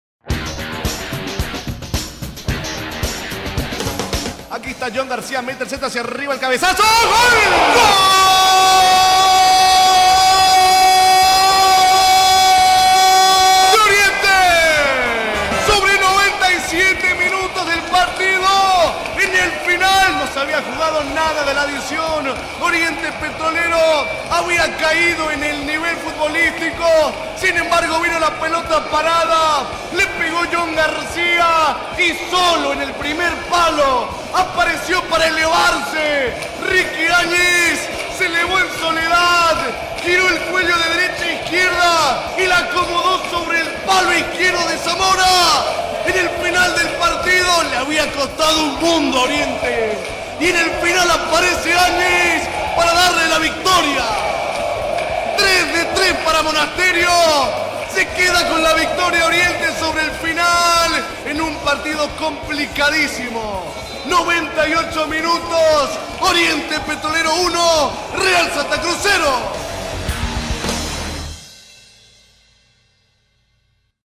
¡TRES PARTIDOS, TRES VICTORIAS! REAL SANTA CRUZ 0-1 ORIENTE PETROLERO
Por el complemento de la fecha 7 en el futbol nacional RealSantaCruz ejercía como local en el Tahuichi Aguilera frente a OrientePetrolero, los refineros se llevaron la victoria sobre el final con gol de Ricky Añez, con este resultado los Albiverdes acumulan así su tercera victoria consecutiva desde el retornó del torneo, podes escuchar el resumen con el relato de Jose Carlo para RadioSport